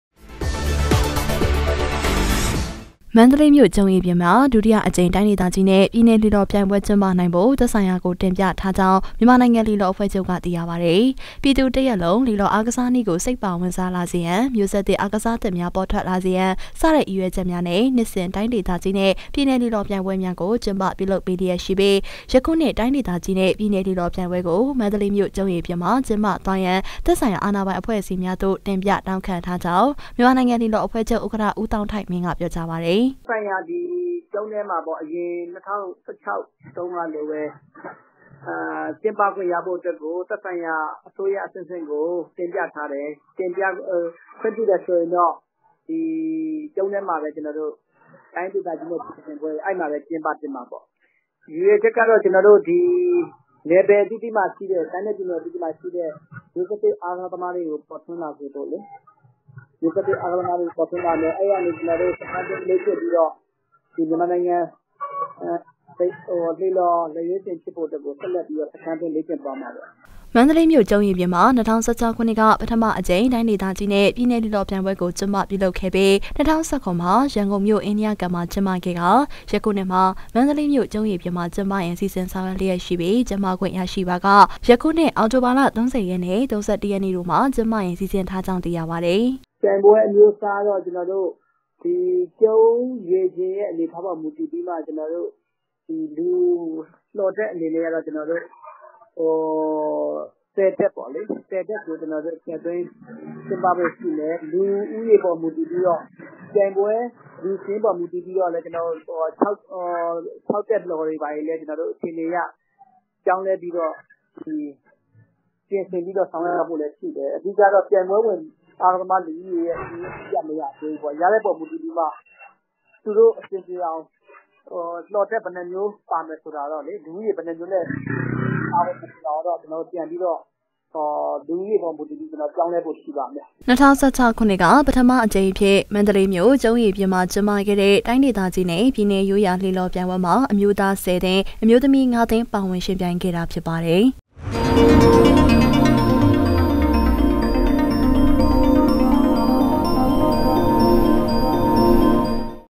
မႏၲေလးၿမိဳ႕က်ဳံးေရျပင္တြင္ဒုတိယအႀကိမ္တိုင္းေဒသႀကီးႏွင့္
မႏၲေလးၿမိဳ႕က်ဳံးေရျပင္တြင္ဒုတိယအႀကိမ္တိုင္းေဒသႀကီးႏွင့္ ျပည္နယ္ေလွေလွာ္ၿပိဳင္ပြဲ က်င္းပႏုိင္ရန္တင္ျပထား
မႏၲေလးၿမိဳ႕က်ဳံးေရျပင္တြင္ ဒုတိယအႀကိမ္တိုင္းေဒသႀကီးႏွင့္ျပည္နယ္ေလွေလွာ္ၿပိဳင္ပြဲက်င္းပႏုိင္ဖုိ႔ သက္ဆုိင္ရာ ကုိ တင္ျပထားေၾကာင္း ျမန္မာႏိုင္ငံေလွေလွာ္အဖြဲ႕ခ်ဳပ္က သိရပါတယ္။